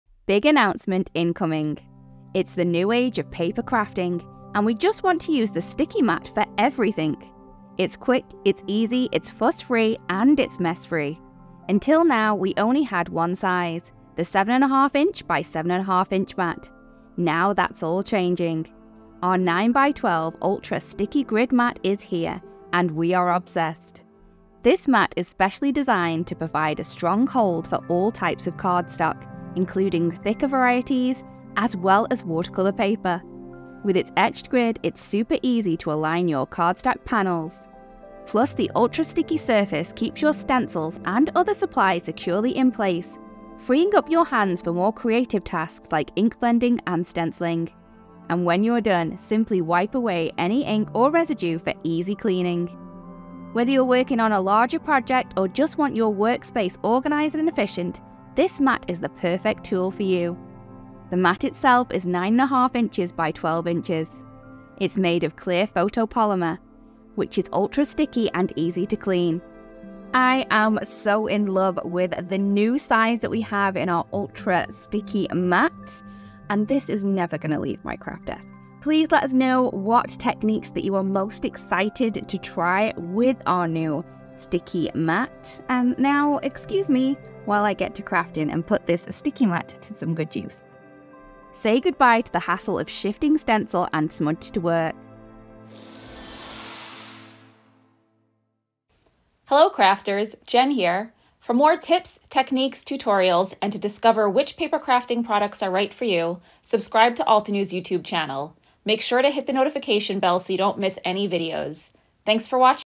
3 Things You Can Do with the Ultra Large Sticky Mat You Couldn't Do Before
It's the new age of paper crafting and we want to use the sticky mat for EVERYTHING. It's quick. It's easy. It's fuss-free. It's mess-free! But until now, we only had one size: the 7.5" x 7.5" mat, but that's all changing.

Watch the video and discover the3 things you can do with our NEW Ultra Large Sticky Mat that you couldn't do with other crafting mats before!

About the Product:
Introducing... our NEW Stampwheel 9"x12" Ultra Sticky Mat with Grid! This mat provides a secure grip for your cardstock and watercolor paper panels, and comes with an etched grid, ensuring you get perfectly aligned, perfectly stamped images and sentiments every time! The ultra-sticky surface also ensures that your stencils and other supplies stay firmly in place so that you can focus on creative tasks like ink blending and stenciling. It's easy to clean, too! After use, simply wipe away any ink or residue.

Whether you're working on large projects or just aiming to keep your workspace organized and efficient, the Stampwheel 9"x12" Ultra Sticky Mat with Grid is the best tool for you!

to learn more about what Altenew can do for YOU! for fun inspiration, prizes, and more!

🛍️ Ready to upgrade your paper crafting game? Start shopping today